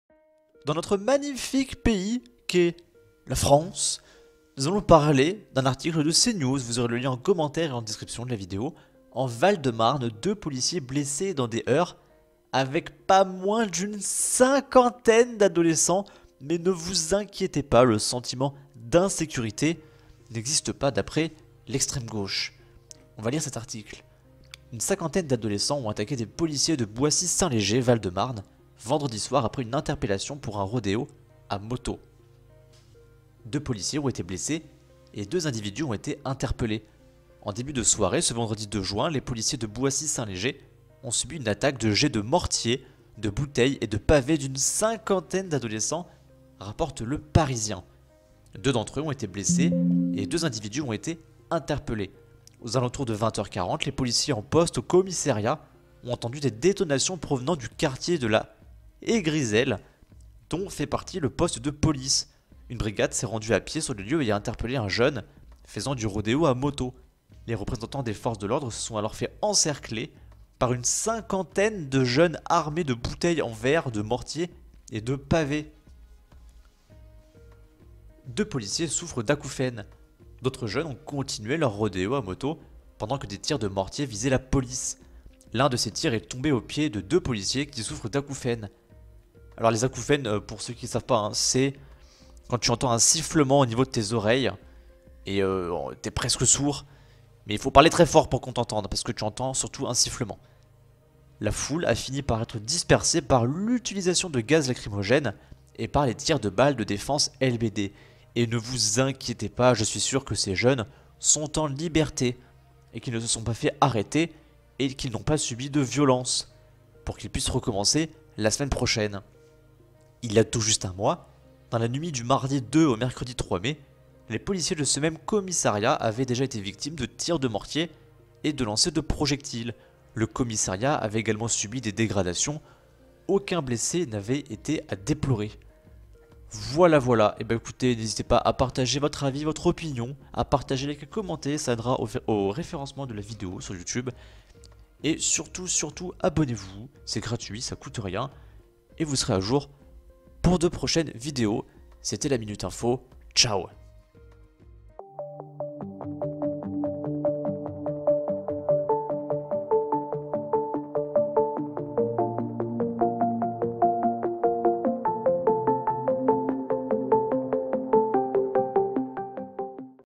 UN COMMISSARIAT ATTAQUÉ PAR 50 PERSONNES EN FRANCE , NE VOUS INQUIÉTEZ PAS , PERSONNE N'A ÉTÉ ARRÊTÉ
J'espère que ces jeunes ne seront pas stigmatisés !

Bon visionnage !